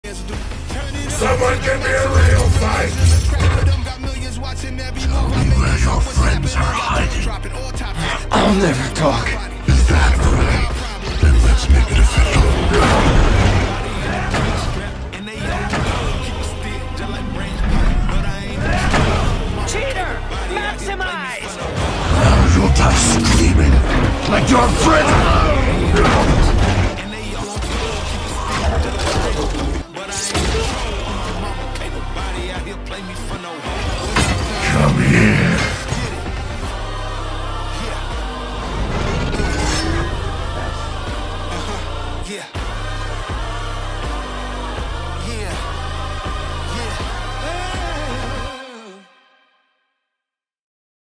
Cheetor Vs. Battletrap Stop Motion | Transformers Rise of the Beasts
Someone give this man a real fight!

Hope this upload does well, I’ve been working on it for a while now and I grinded out the last half of the video today as of uploading it. If you’ve read this far, comment “Bean and Cheese.”